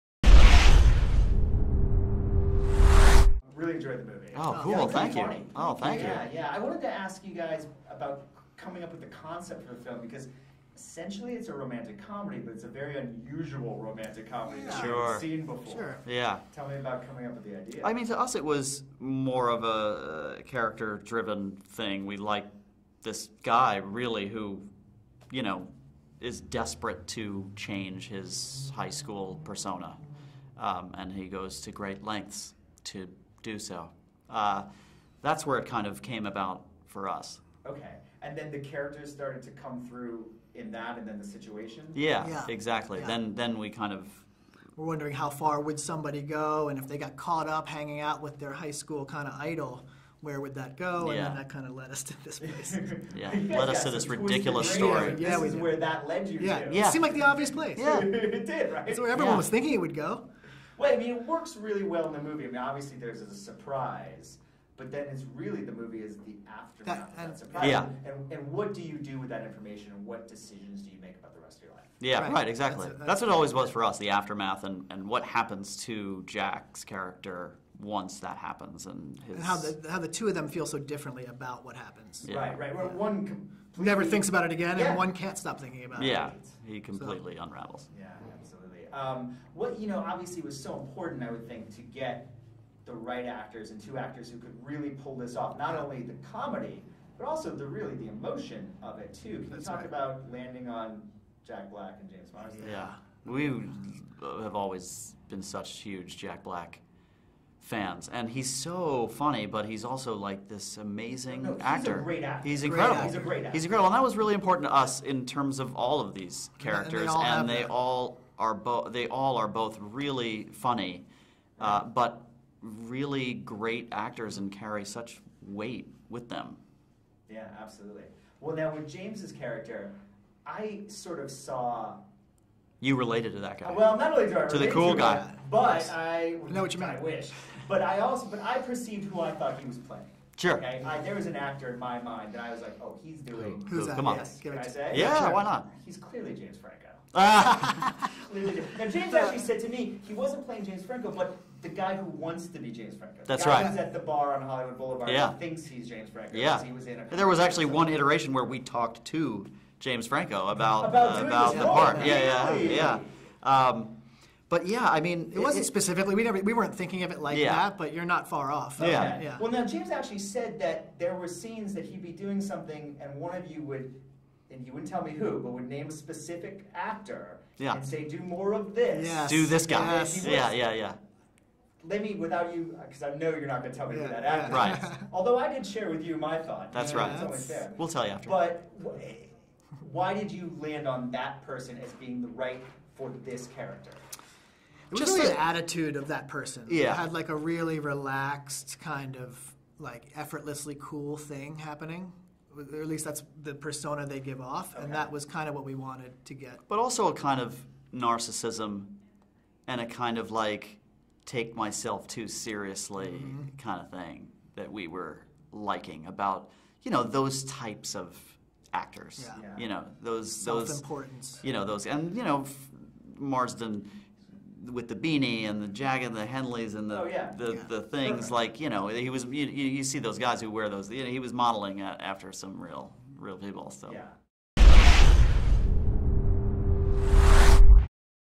Andrew Mogel and Jarrad Paul
Jami Philbrick speaks with writers and directors Andrew Mogel and Jarrad Paul about their new film 'The D Train,' which stars Jack Black and opens in theaters on May 8th.